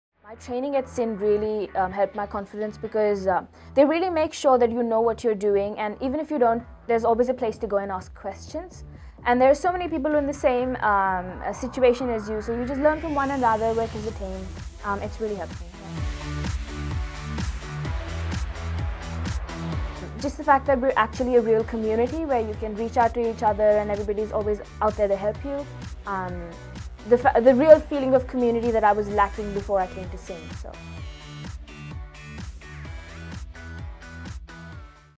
Vrinda shares her SYN experience
Vrinda from 'Wanderlust Melbourne' (part of our International Students on Air training) shares her experience at SYN.